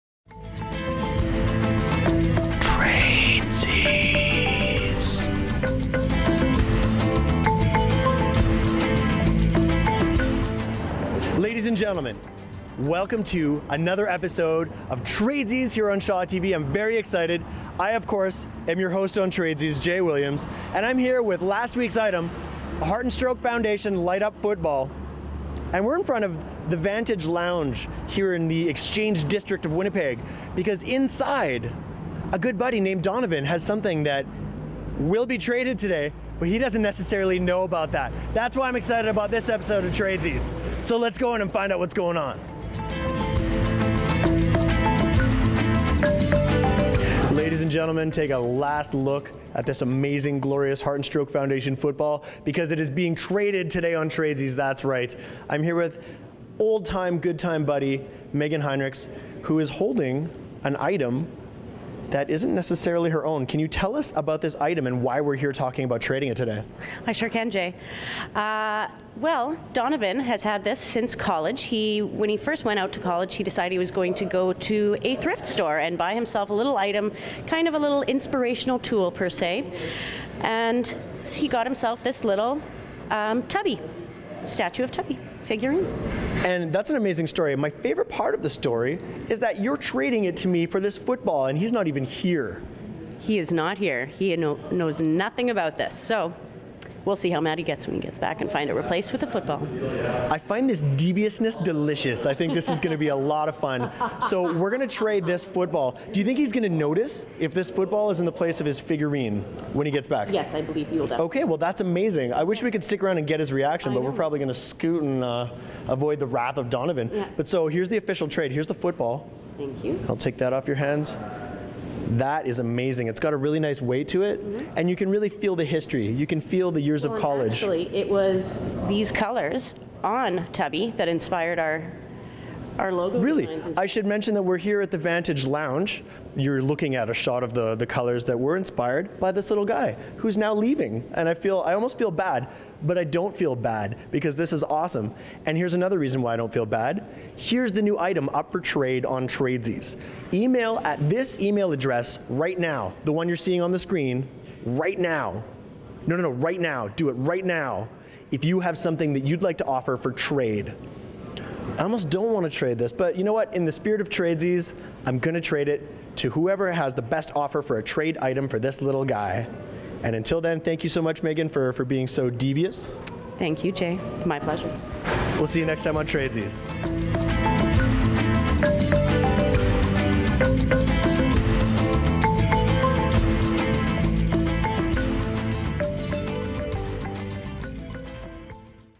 Tradesies - Episode 3
J.Williams trades a fancy light-up football for a vintage figurine, and has a really good time doing it.